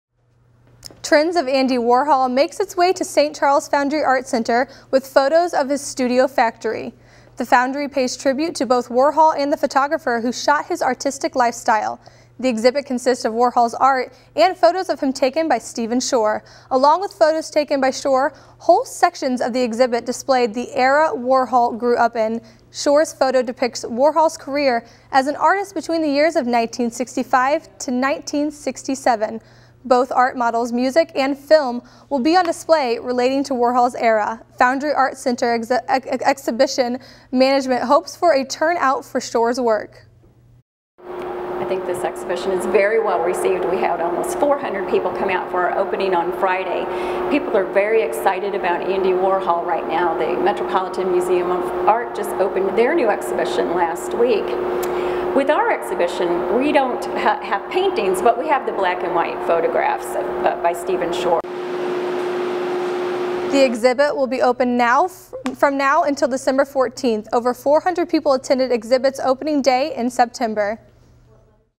The Velvet Years
LUTV News explores the new Andy Warhol exhibit called The Velvet Years at the Foundry Art Center. The exhbit takes a look at Warhol's factory with photographs by Stephen Shore from 1965 to 1967.